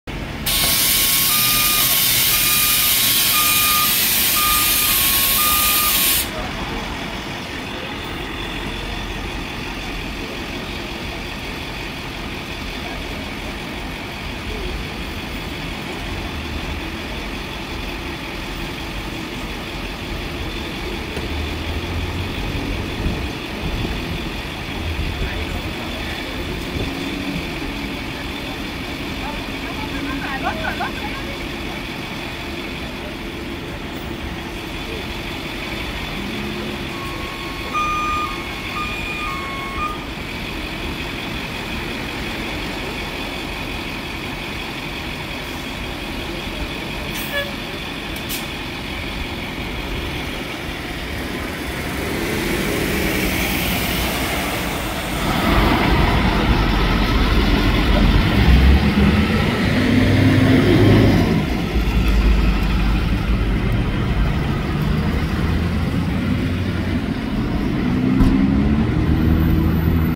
6517 on the 22 (Hoboken)
Former Coach USA one bus now an NJ TRANSIT Meadowlands Bus. On Bergenline Av & 62nd St in WNY; Hoboken bound.